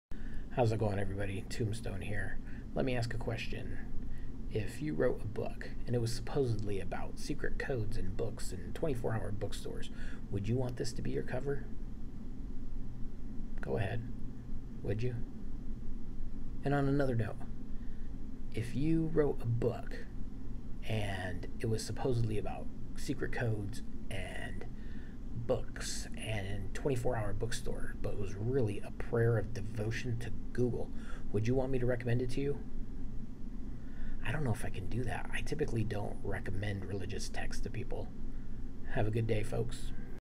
Mr. Penumbra’s 24-Hour Bookstore—Religious Zealotry?
Tombstone’s short little rant about Mr. Penumbra’s 24-Hour Bookstore, a book that is religious devotion to Google.